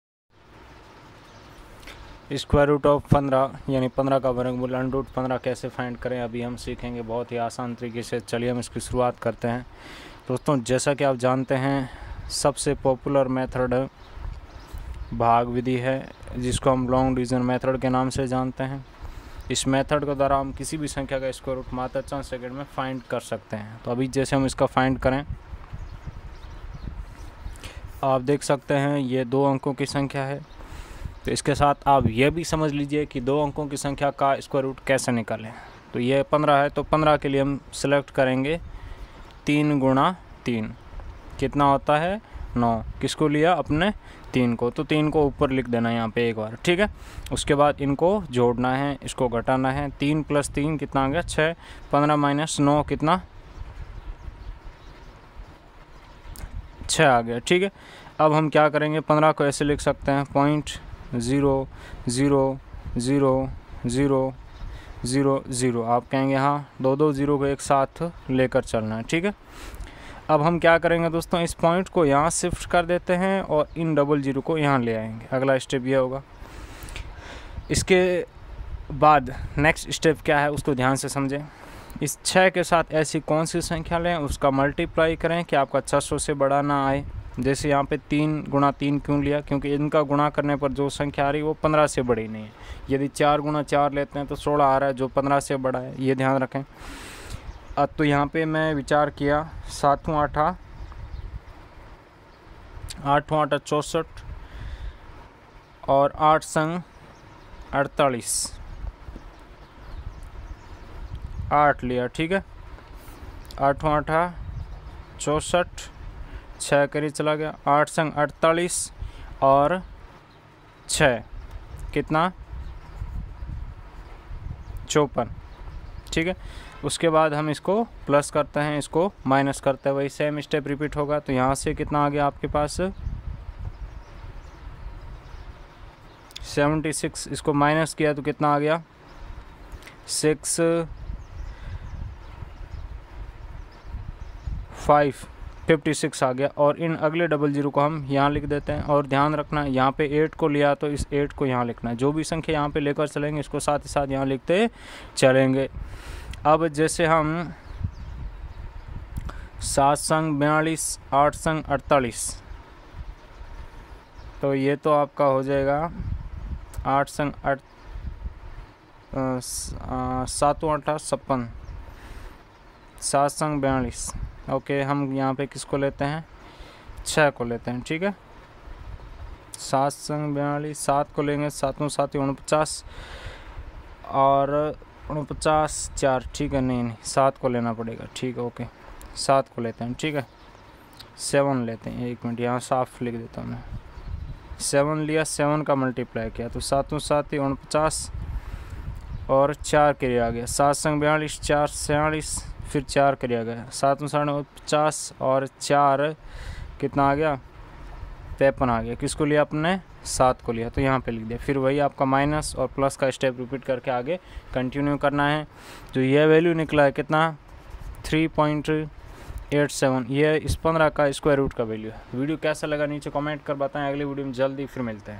√15 | Square Root of 15 in Hindi | Maths By KclAcademy | वर्गमूल निकालना In 3 Minutes | हिंदी में
find square root of 15 | How to find square roots using long division method | √15